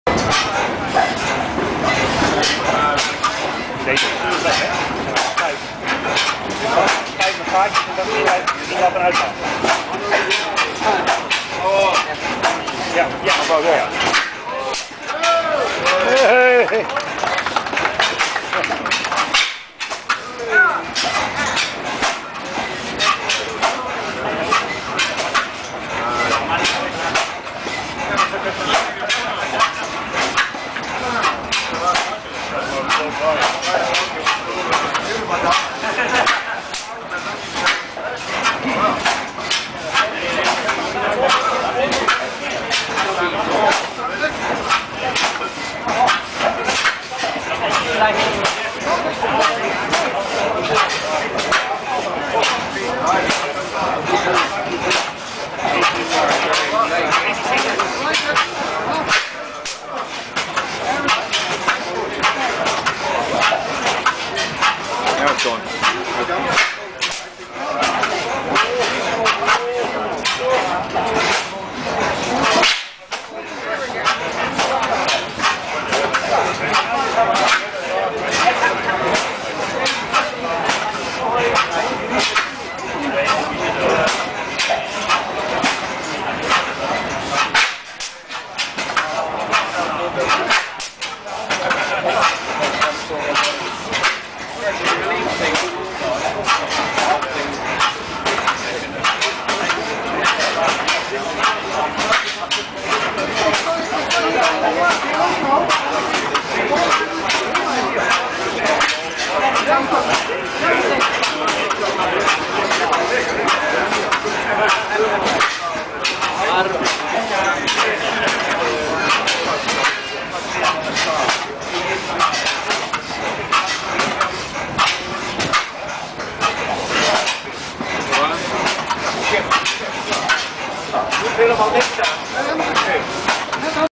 1860 Lenoir running. At the 2013 Nuenen Engine Rally - On special loan from the Deutz Collection
1860 Lenoir running.  At the 2013 Nuenen Engine Rally - On special loan from the Deutz Collection
Running on synthetic illuminating gas - electric spark ignition